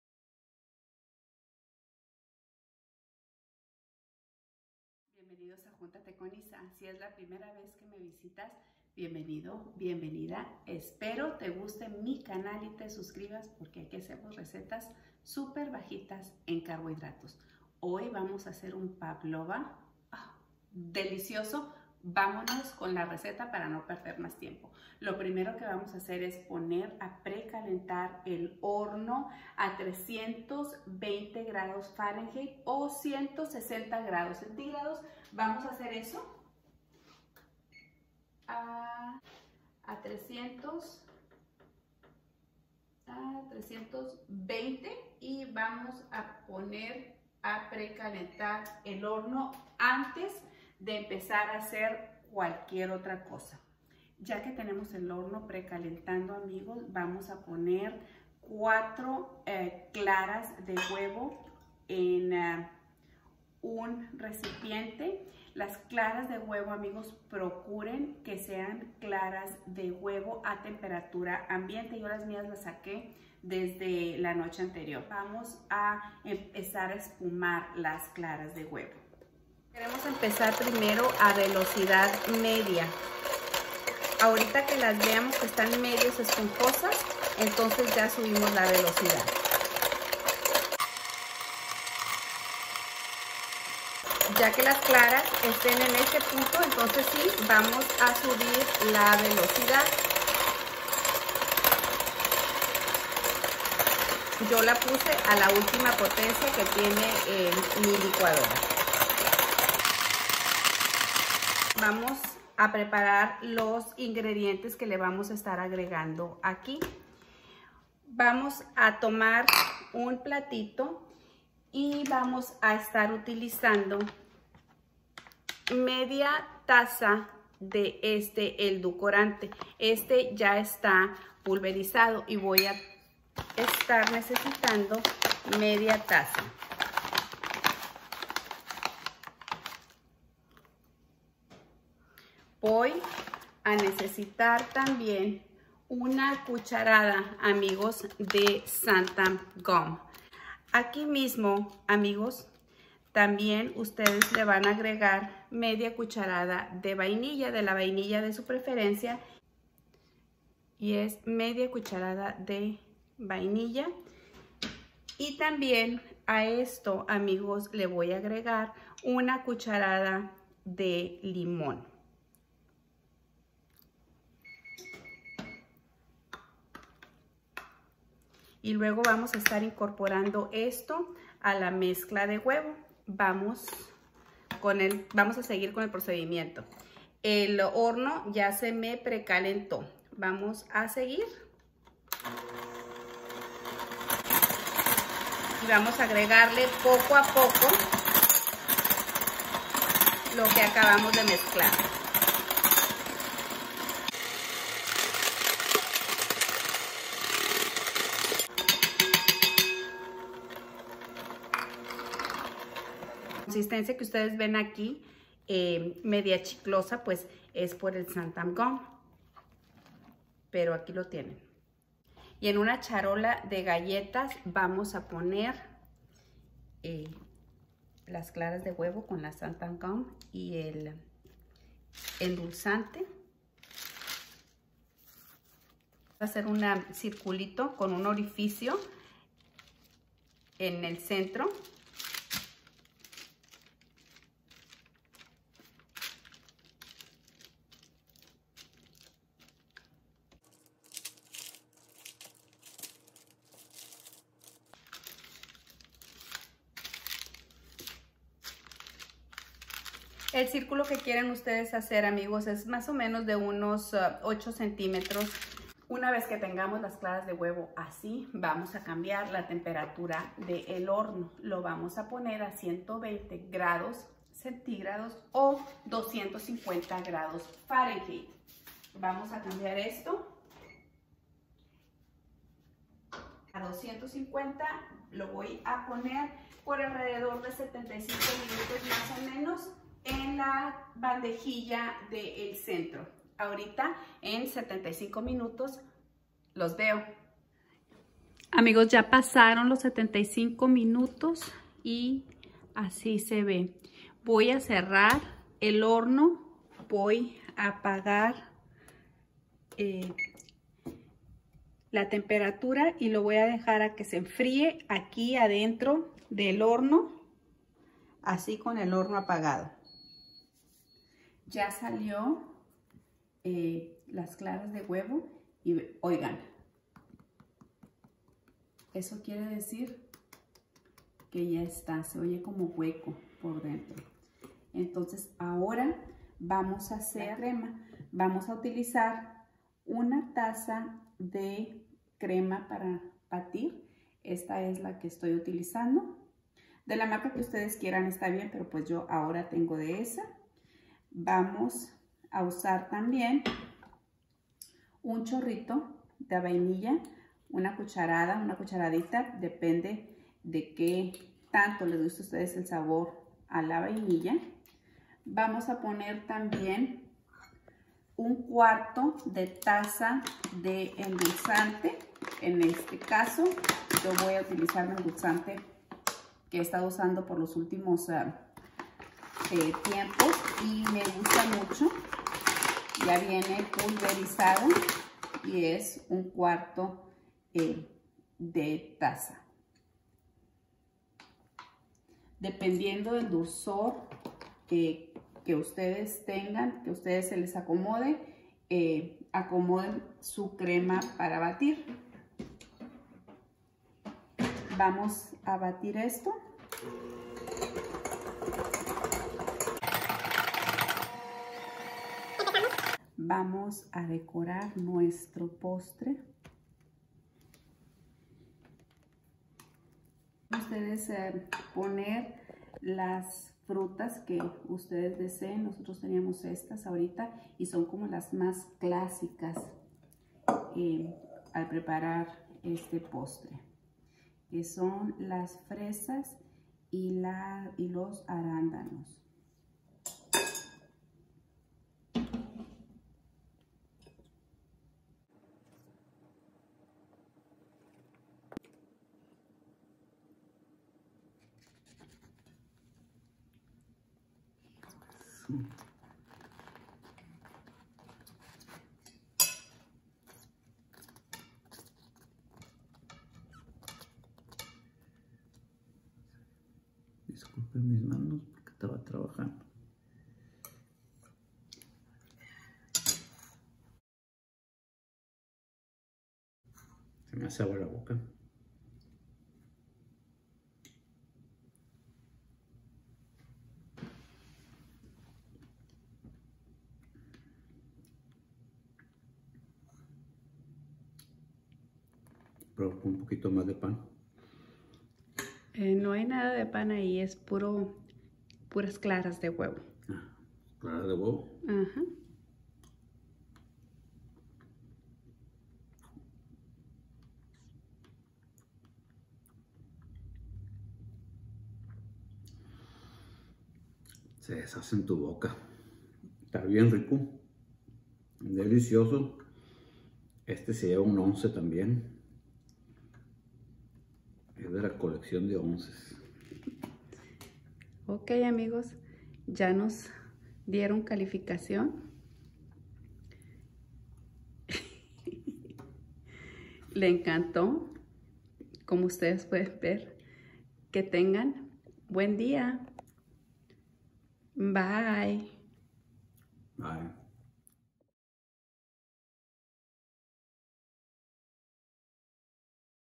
Pavlova bajo en carbohidratos
Te sobran 4 gramos de carbohidratos y quieres un postre delicioso? Te invito a que prepares este riquísimoPavlova con  muy poquitos ingredientes y súper faciel de preparar pero sobretodo es DELICIOSO
Aquí está mi versión Keto de un Pavlova

👩‍🍳 Ingredientes
4 claras de huevo a temperatura ambiente
1/2 taza de endulzante pulverizado
1 cucharadita chica de goma Xanthana
1/2 cucharada sopera de vainilla
1 o 2 cucharadas soperas de limón
👩‍🍳Para el relleno
1 taza de crema para batir
1 cucharada grande de vainilla
1/4 taza de endulzante pulverizado
1 t de fresas
1/2 t de arándanos  o blueberries

🎯Macros si lo divides en 6 porciones
4g de carbohidratos
15g de grasa
4g de proteínas
171g calorías

y en Instagram @Juntate con Isa